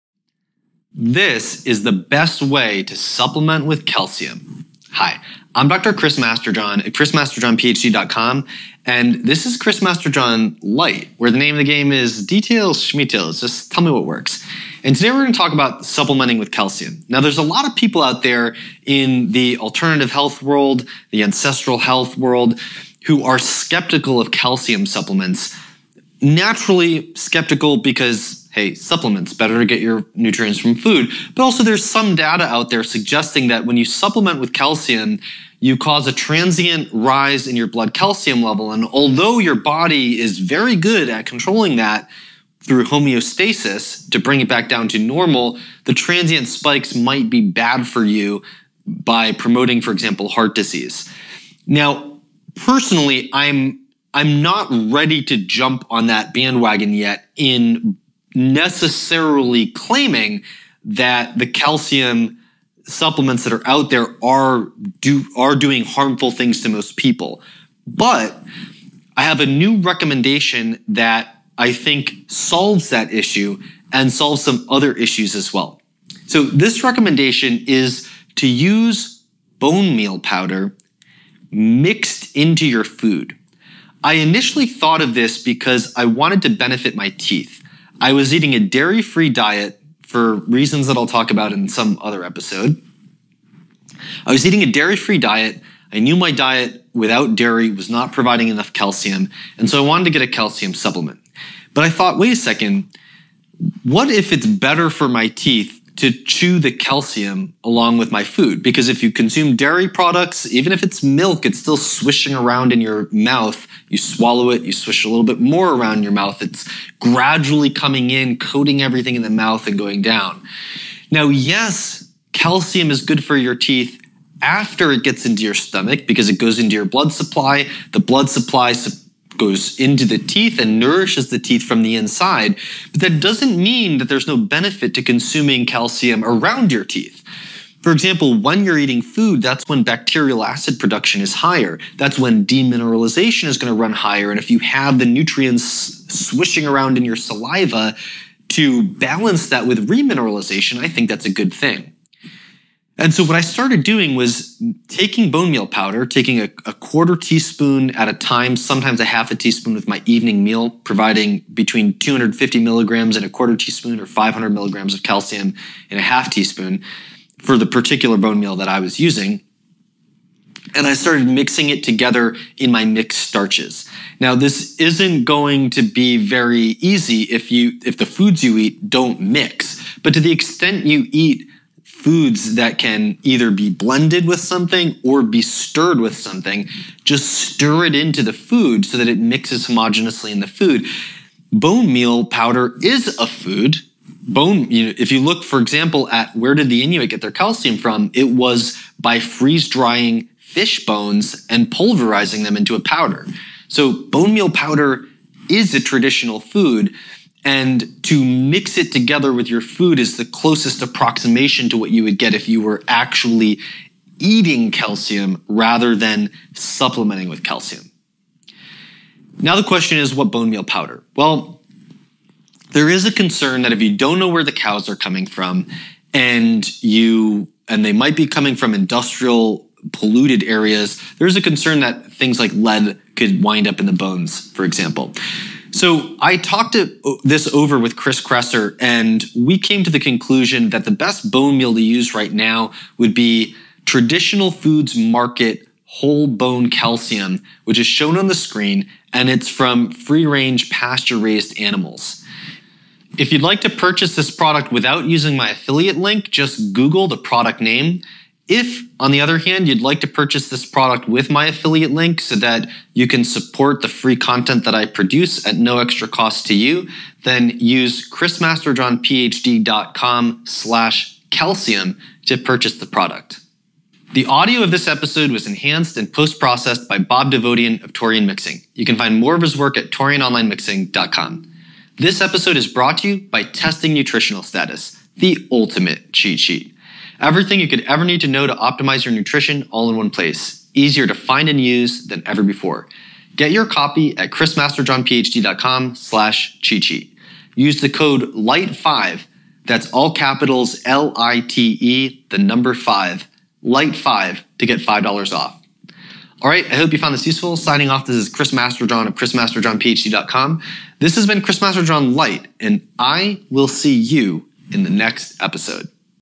The Best Way to Supplement With Calcium | Chris Masterjohn Lite # 58
Chris Masterjohn, PhD is the Founder and Scientific Director of the mitochondrial test mito.me. Calcium supplementation is controversial. Some people believe that supplements, but not calcium-rich foods, can increase the risk of heart disease. Here are my recommendations for calcium supplementation to make it as close to eating whole foods as possible.

The bone meal powder I use can be found .